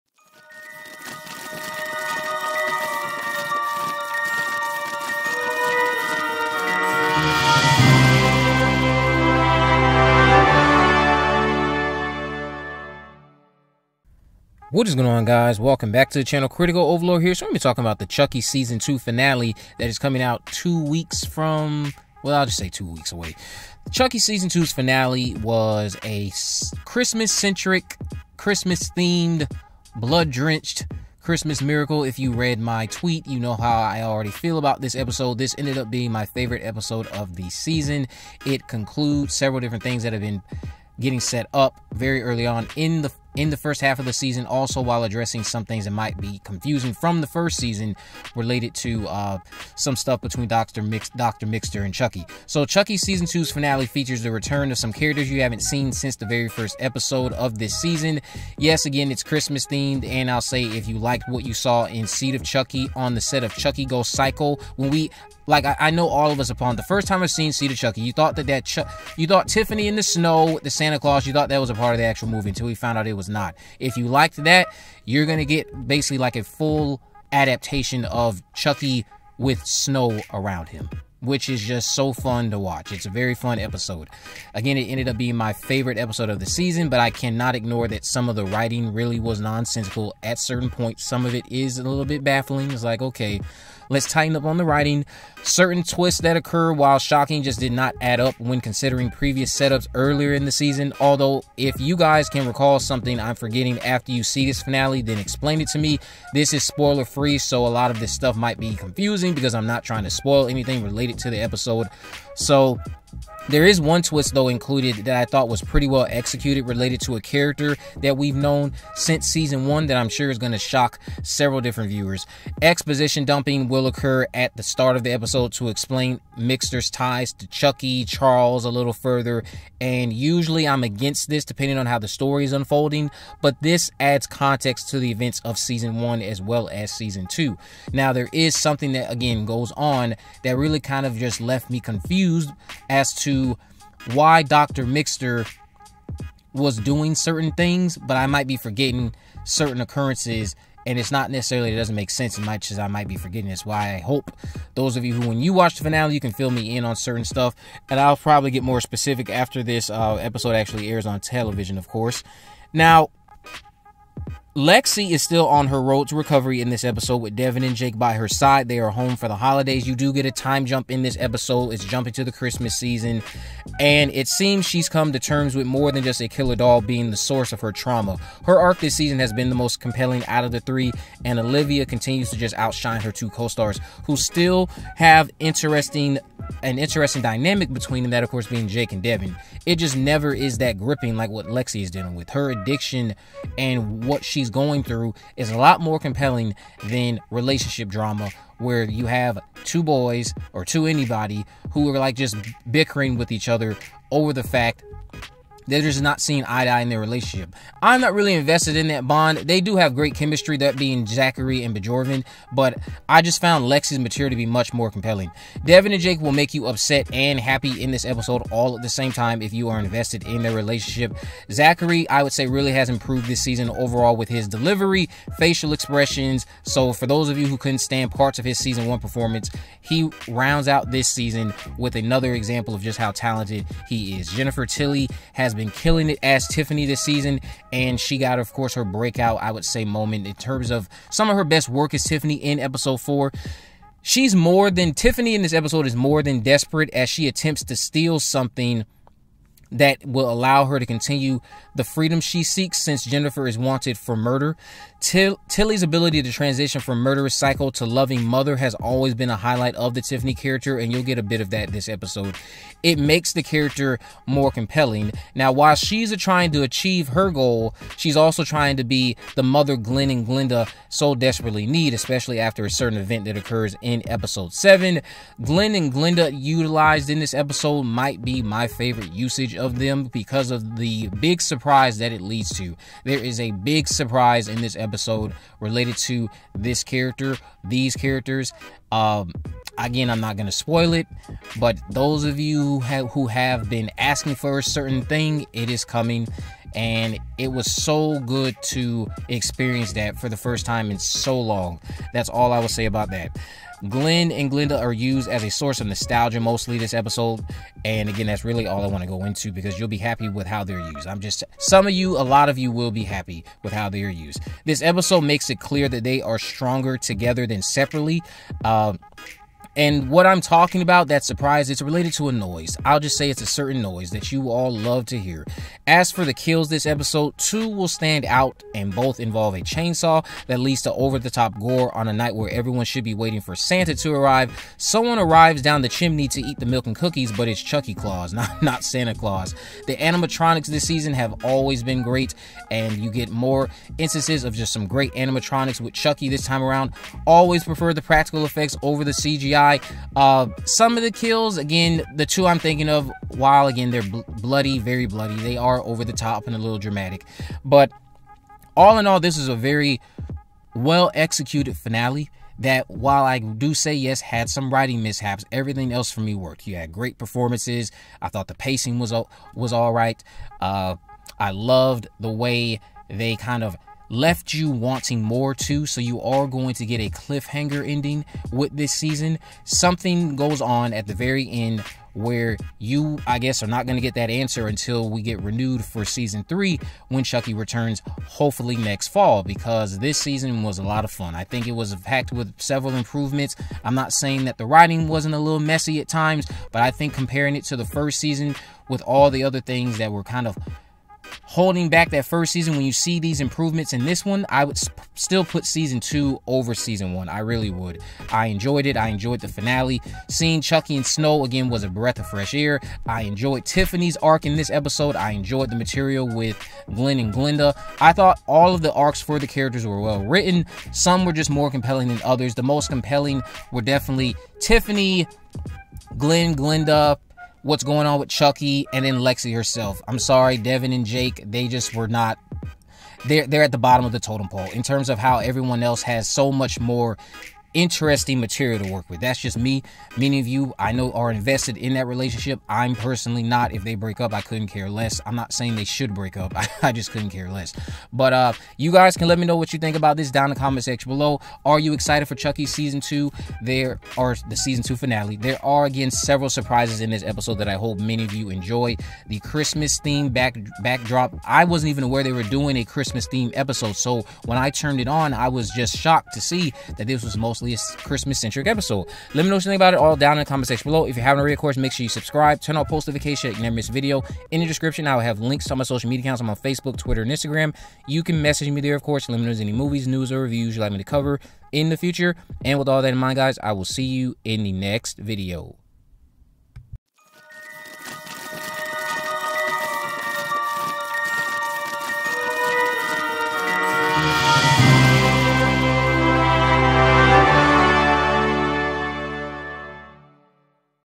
CHUCKY TV Series | Season 2 - Episode 8 Review
Chucky Season 2 is off to a strong start! I've seen the first 3 episodes of the upcoming series. Season 2 follows Chucky stalking Lexy, Devon, and Jake at Incarnate Lord away from Hackensack. Don Mancini's pint-sized killer doll is back on USA/Syfy this fall!